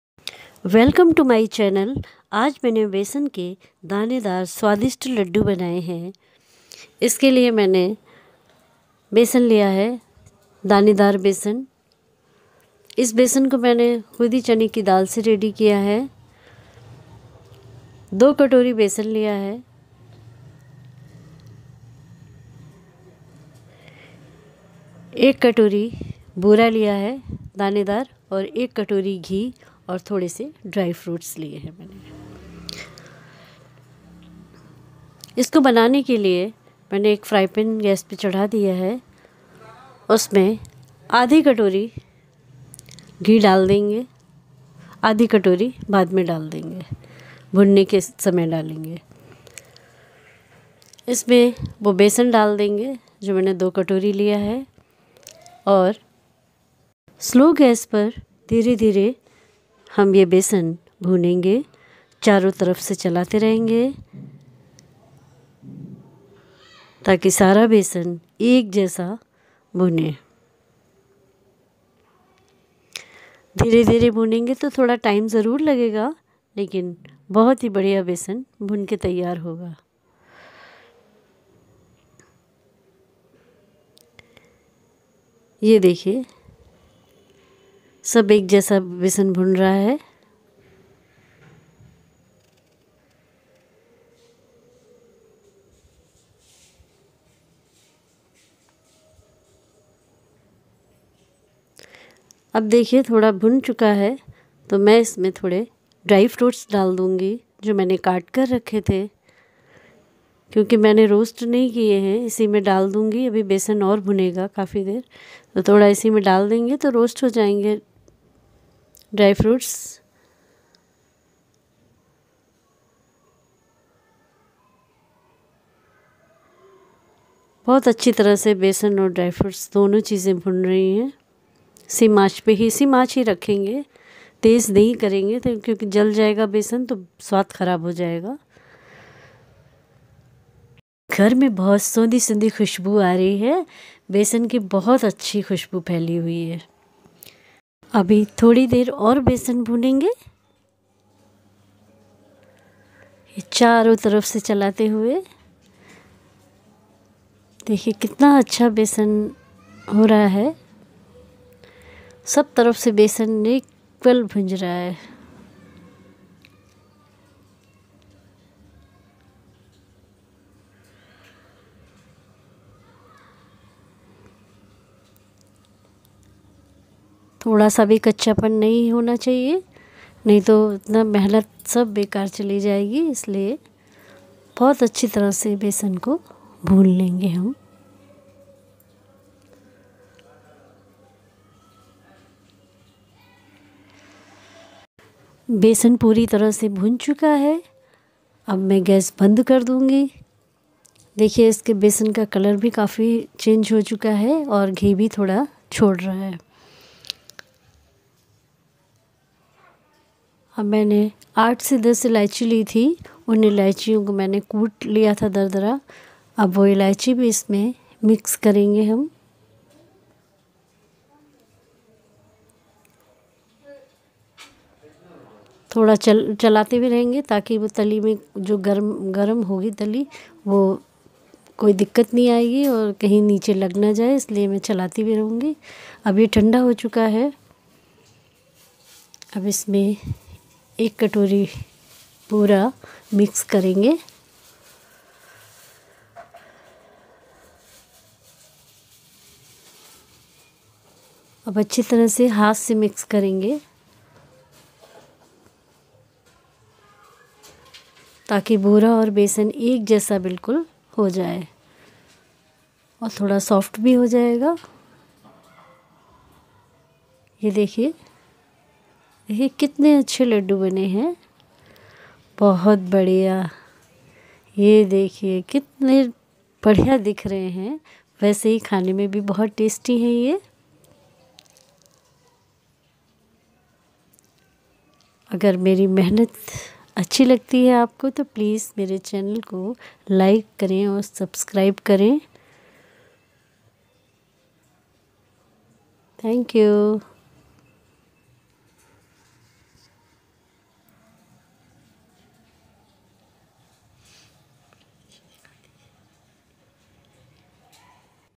Besan ke laddoo recipe l Neelam's little cook book
danedar besan  - 2 bowls
bura - 1 bowl
ghee - 1 bowl
ilaichi - 8 to 10
dry fruits - 2 spoons